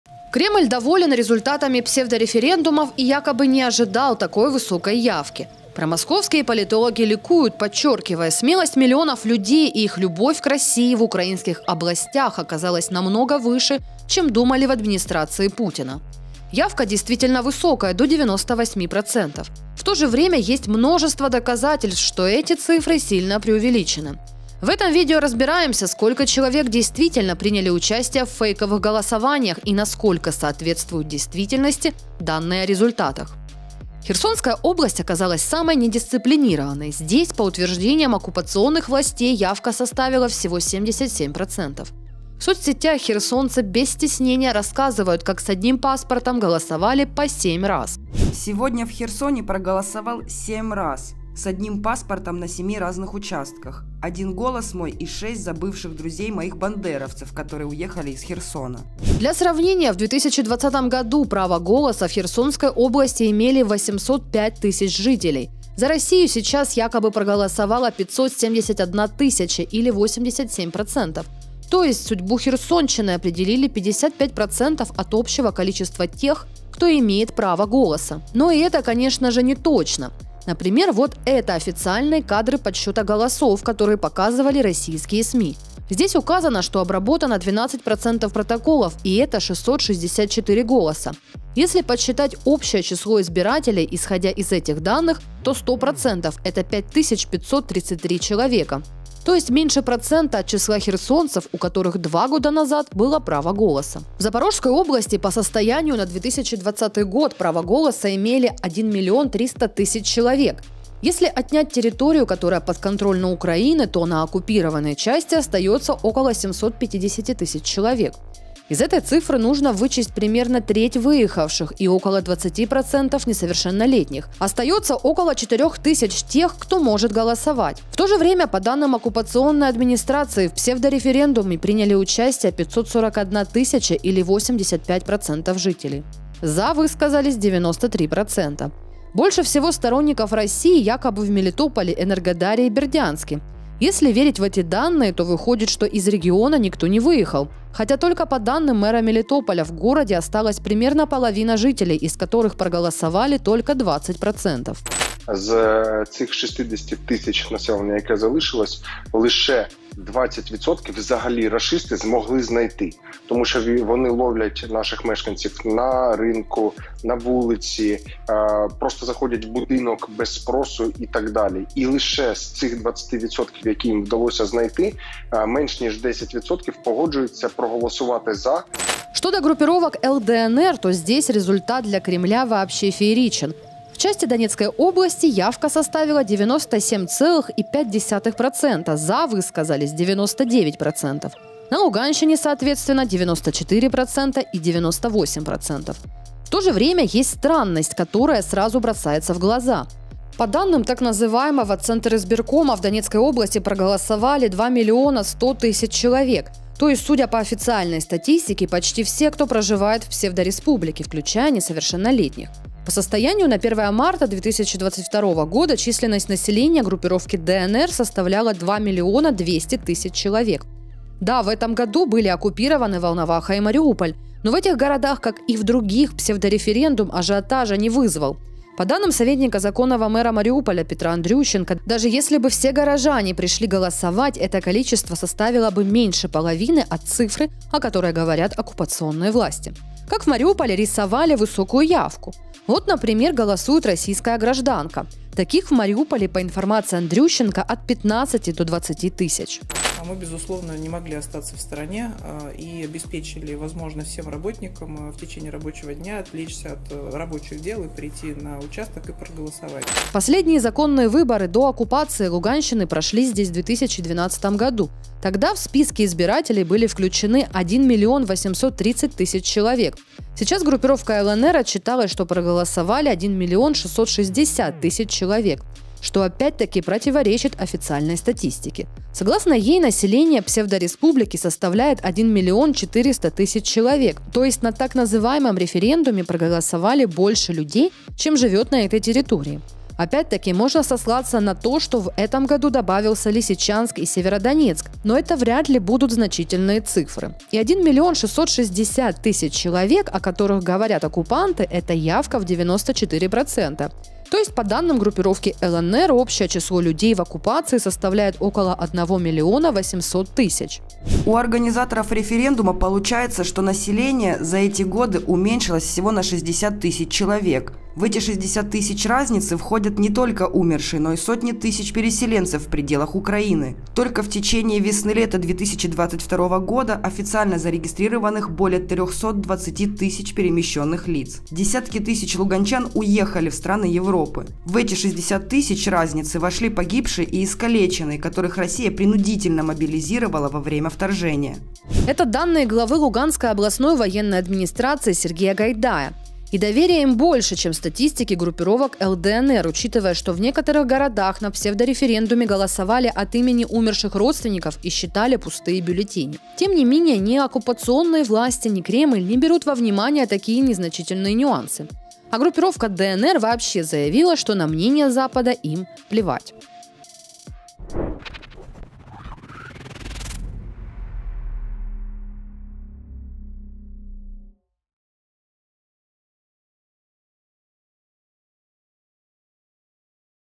Сколько человек реально проголосовали на "референдумах"?
Кремль доволен результатами псевдореферендумов и якобы не ожидал такой высокой явки. Промосковские политологи ликуют, подчеркивая: смелость миллионов людей и их любовь к России в украинских областях оказались намного выше, чем думали в администрации Путина. Явка действительно высокая - до 98%. В тоже время есть множество доказательств, что эти цифры сильно преувеличены. В этом видео разбираемся, сколько человек действительно приняли участие в фейковых голосованиях и насколько соответствуют действительности данные о результатах?

Херсонская область оказалась самой недисциплинированной. Здесь, по утверждениям оккупационных властей, явка составила всего 77%.

Для сравнения в 2020 году право голоса в Херсонской области имели 805 тысяч жителей. За Россию сейчас якобы проголосовала 571 тысяча или 87%. То есть, судьбу Херсонщины определили 55% от общего количества тех, кто имеет право голоса. Но и это, конечно же, не точно. Например, вот это официальные кадры подсчета голосов, которые показывали российские СМИ. Здесь указано, что обработано 12% протоколов и это 664 голоса. Если подсчитать общее число избирателей, исходя из этих данных, то 100% - это 5533 человека. То есть, меньше процента от числа херсонцев, у которых 2 года назад было право голоса.

В Запорожской области по состоянию на 2020 год право голоса имели 1,3 миллиона человек. Если отнять территорию, которая подконтрольна Украине, то на оккупированной части остается около 750 тысяч человек. Из этой цифры нужно вычесть примерно треть выехавших и около 20% несовершеннолетних. Остается около 400 тысяч тех, кто может голосовать. В тоже время, по данным оккупационной администрации, в псевдореферендуме приняла участие 541 тысяча или 85% жителей. “За” высказались 93%. Больше всего сторонников России якобы в Мелитополе, Энергодаре и Бердянске. Если верить в эти данные, то выходит, что из региона никто не выехал. Хотя, только по данным мэра Мелитополя, в городе осталась примерно половина жителей, из которых проголосовали только 20%.

Что до группировок “ЛДНР”, то здесь результат для Кремля вообще фееричен. В части Донецкой области явка составила 97,5%, “за” высказались 99%. На Луганщине соответственно 94% и 98%. В тоже время есть странность, которая сразу бросается в глаза. По данным так называемого “центризбиркома”, в Донецкой области проголосовали 2,1 миллиона человек. То есть, судя по официальной статистике, почти все, кто проживает в псевдореспублике, включая несовершеннолетних. По состоянию на 1 марта 2022 года численность населения группировки “ДНР” составляла 2,2 миллиона. Да, в этом году были оккупированы Волноваха и Мариуполь, но в этих городах, как и в других, псевдореферендум ажиотажа не вызвал. По данным советника законного мэра Мариуполя Петра Андрющенко, даже если бы все горожане пришли голосовать, это количество составило бы меньше половины от цифры, о которой говорят оккупационные власти. Как в Мариуполе рисовали высокую явку? Вот, например, голосует российская гражданка. Таких в Мариуполе, по информации Андрющенко, от 15 до 20 тысяч.